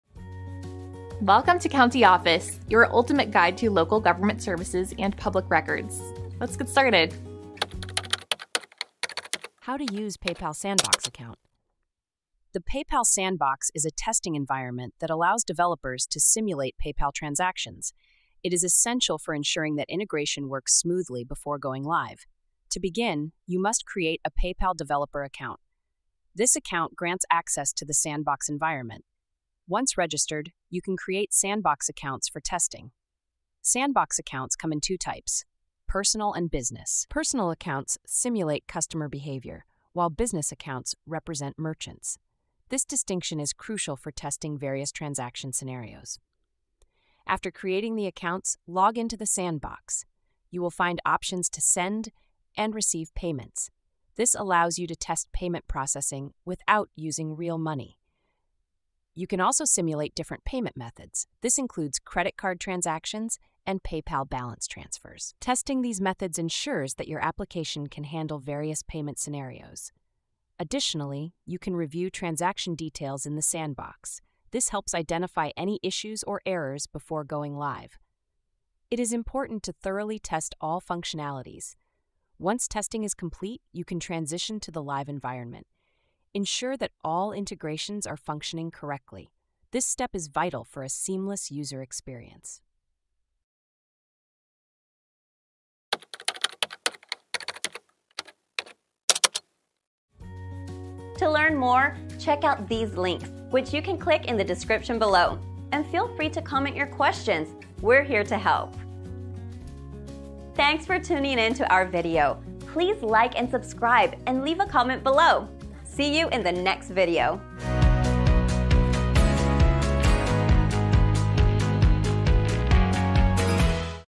How To Use PayPal Sandbox Account? - CountyOffice.org
In this detailed video, we explore the ins and outs of using a PayPal Sandbox account for testing transactions before going live. The PayPal Sandbox serves as a crucial testing ground for developers to ensure seamless integration and functionality. By creating Sandbox accounts, both personal and business, users can simulate various transaction scenarios without the use of real money. This allows for thorough testing of payment processing and different payment methods, including credit card transactions and PayPal balance transfers.

Furthermore, users can review transaction details within the Sandbox to identify and address any potential issues or errors before transitioning to the live environment. Proper testing of all functionalities is essential for a smooth user experience and successful integration.